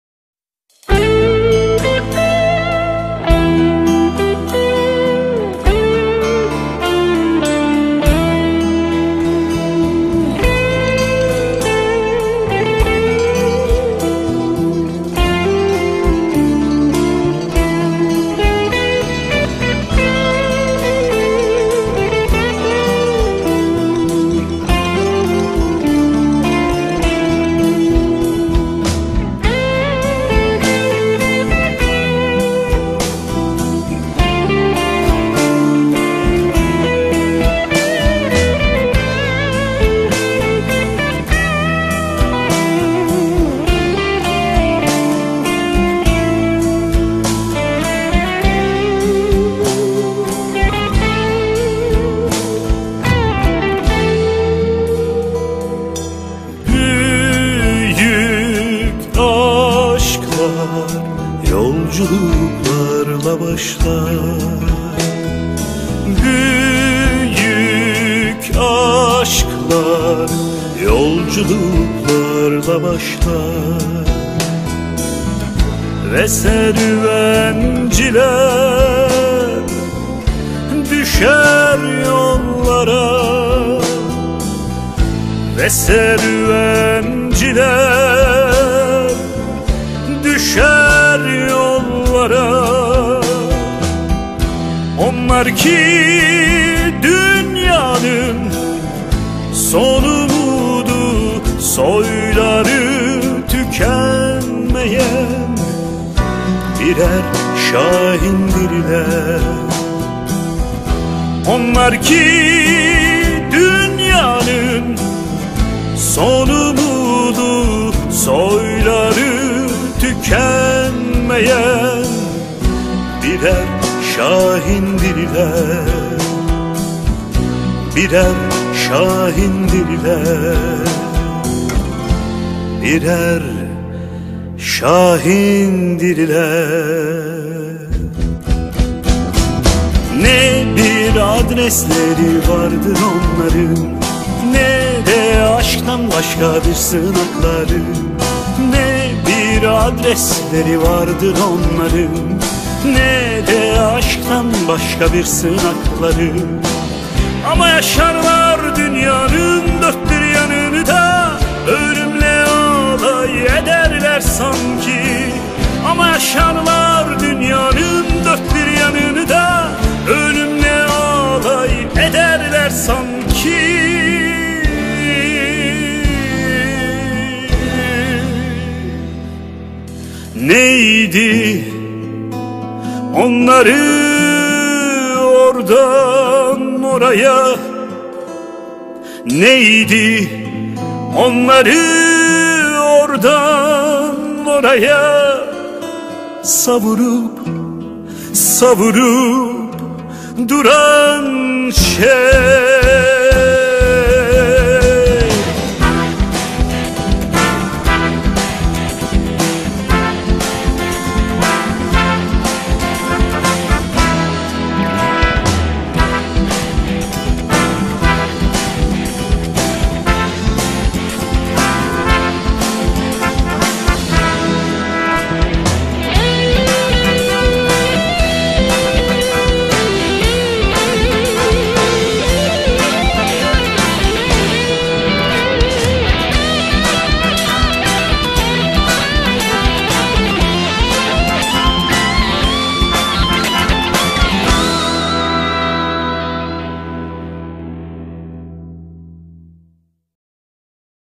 Soluk Soluğa-Efkan Şeşen (Ahmet Telli)
SOLUK SOLUĞA-2

Büyük aşklar yolculuklarla başlar
ve serüvenciler düşer bu yollara ancak

Onlar ki dünyanın son umudu
soyları tükenen birer çılgındırlar

Ama yaşarlar dünyanın dört bir yanında
Ölümle alay ederler sanki

Nerde beklenirse ordaydılar
bir kez bile gecikmediler ömür boyu

Neydi onları ordan oraya
savurup duran şey

Onları daima yalnız kılan
neydi bu yaşam denilen gürültüde

Her dilden bir adları vardı onların
ama hiçbir ülkenin kimliğini taşımadılar

Sarışındılar belki de esmer
yani birçok yüzün bileşkesi

Ne altın arayıcısıydılar
ne de aylak bir gezgin

Vurulup düşseler de her kuşatmada
serüvencidir onlar ve hiç ölmezler

Ki onlar hep yalnızdır ve her nasılsa
Bulurlar heder olmanın bir yolunu

Onlar ki bu dünyada
kahraman olmaya mahkumdurlar

Sislenen anılar kaldı bize onlardan
renkleri bozulup duran solgun anılar

Nasıl yazmalı ki silinip gitmesin
bulutlar gibi çekilmesin gök boşluğuna

Bileği güçlü ve gözüpek avcılar mıydı
onları kuşatıp yeryüzü cennetinden atan

Yoksa kendini tüketen hüzünler miydi
vurulup düştükçe ışığını karartan

O serüvenlerin günlüğü tutulmadı
yazılmadı o insanların destan şiiri

Parça parça ettirilseler bir kartala
(ki sanırım böyle oldu sonları)

Fışkırır yüreklerinden
başarısız ihtilallerin yangınları

Ahmet TELLİ